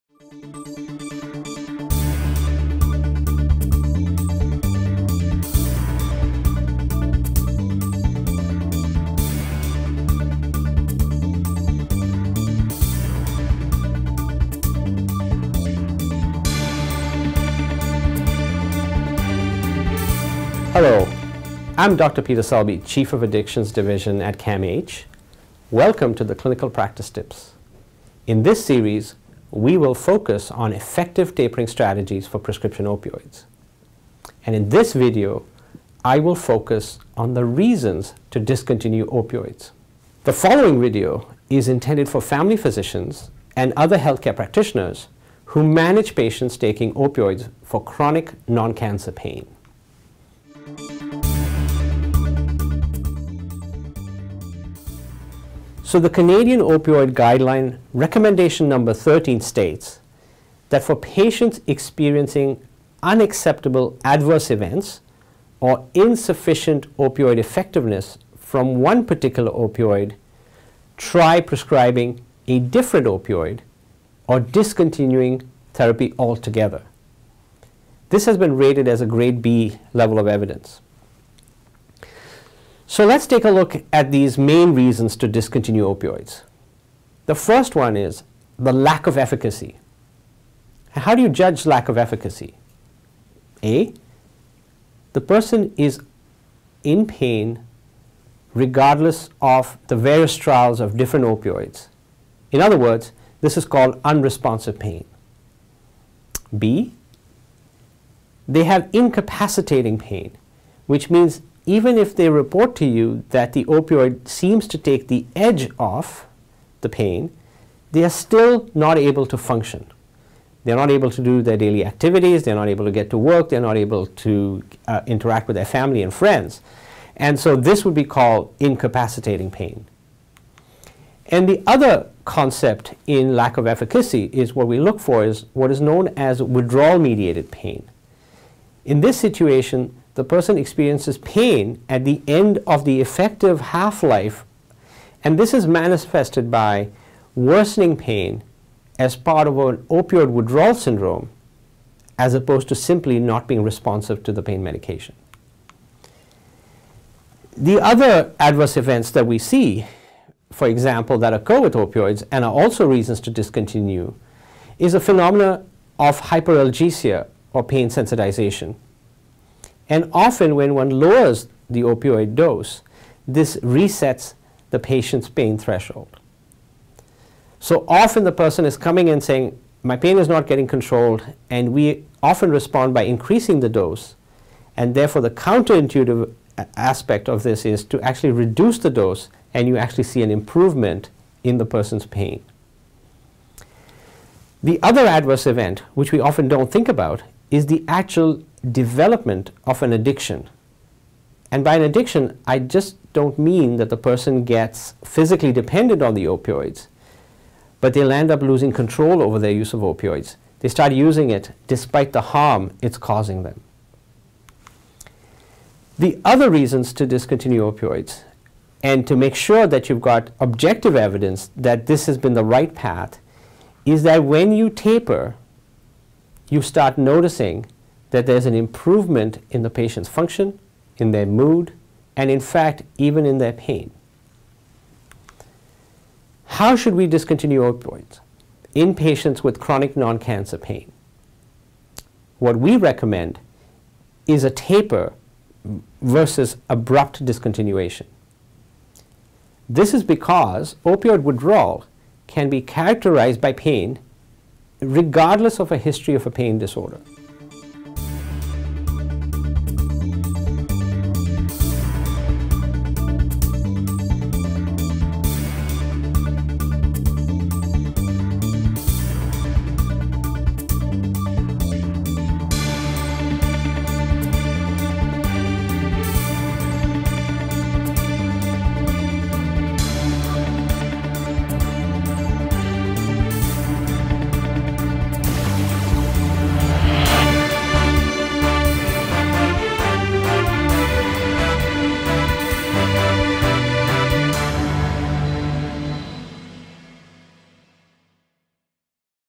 The main reasons to discontinue opioids
Clinical Practice Tips videos: Effective Tapering Strategies for Prescription Opioids
In this series of four videos for family physicians and other health care practitioners, Dr Peter Selby, Chief of the Addictions Division at CAMH, presents guidance on tapering opioids from the Canadian Guidelines for Safe and Effective Use of Opioids for Chronic Non-Cancer Pain.